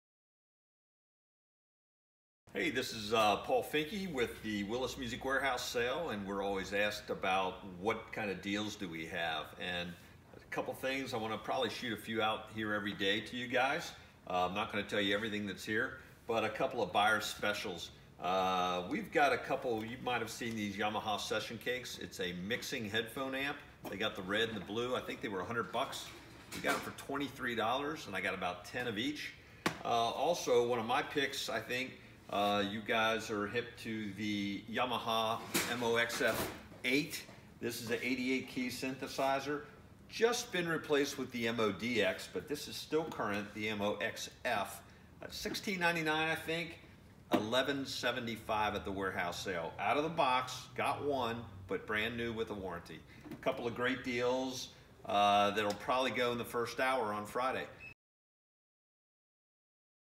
Warehouse Sale Product Preview! The Yamaha MOXF!
It is time for our 17th annual warehouse sale! We are always asked what kind of deals we will have so we are going to spotlight a few this week! Check out VP Paul Finke telling you a little about the deals we have.